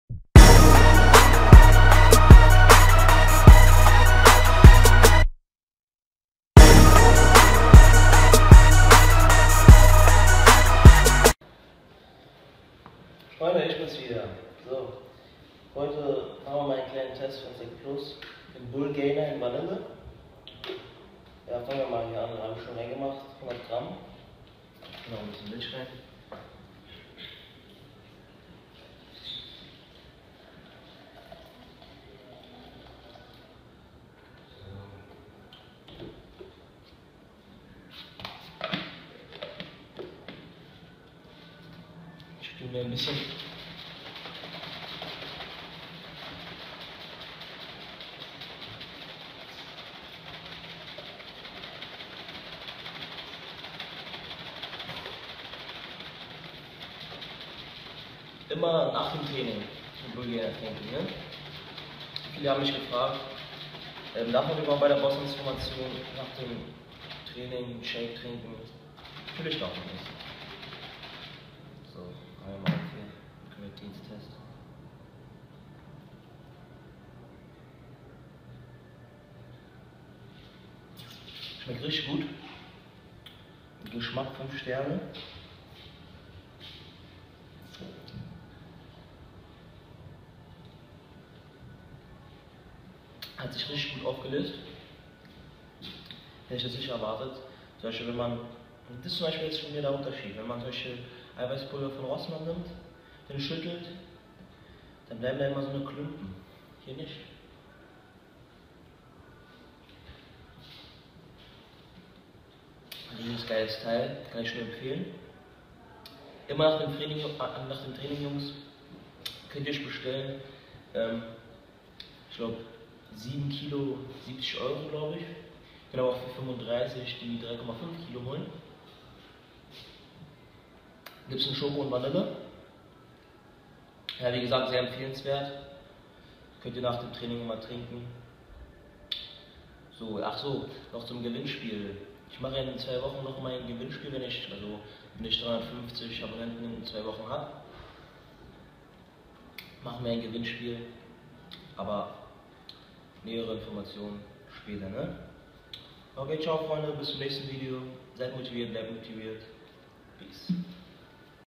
Supplement Test Zec+ Bullgainer
Sehr gute Qualität der Bullgainer kann ich nur empfehlen Freunde.
Kauftst euch bewirkt wunder in der Massenphase und in der Definitionsphase.

Seid Motiviert bleibt Motiviert !